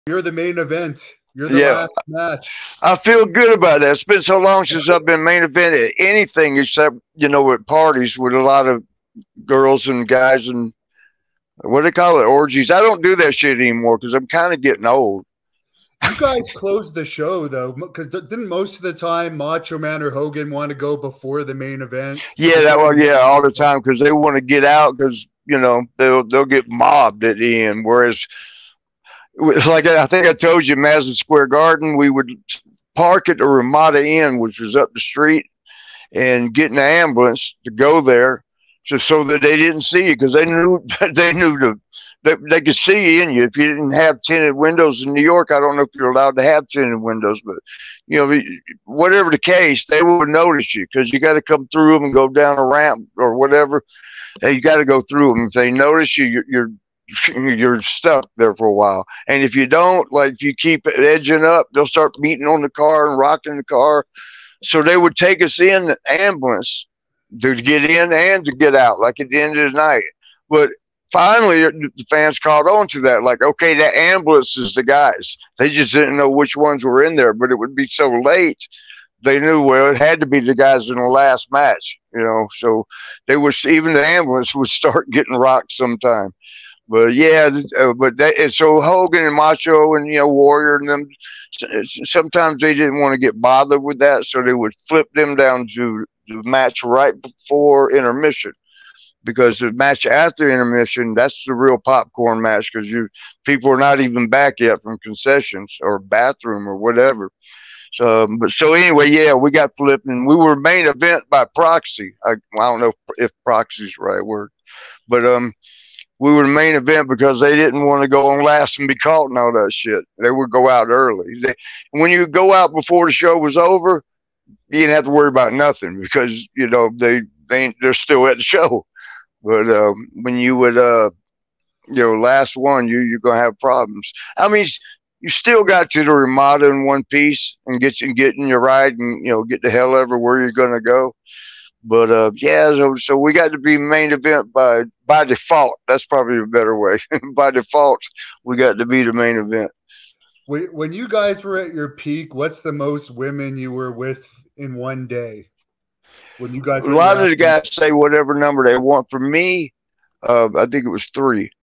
Marty Jannetty on Main Eventing Hogan Headlined WWF Shows
2nd YouTube Channel @ The Hannibal TV FIGHT & UFO VIDEOS